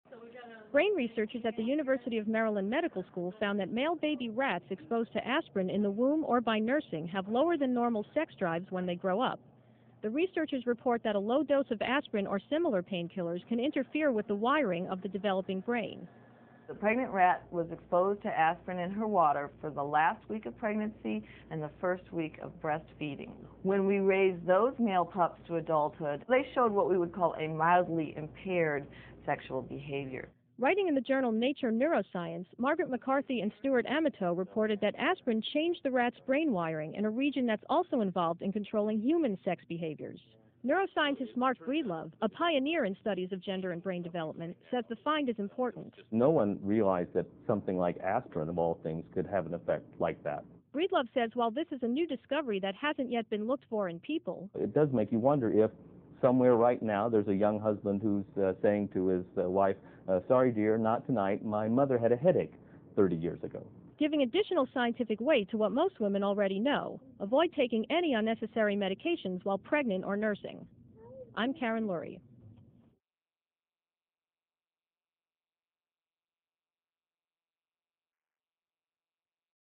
Aspirin & Sex Drive: ScienCentral News Video
A MAN'S SEX DRIVE IN ADULTHOOD COULD BE WEAKENED IF HIS MOTHER TOOK ASPIRIN WHILE PREGNANT. THAT'S THE SUGGESTION OF A NEW STUDY DONE IN RATS.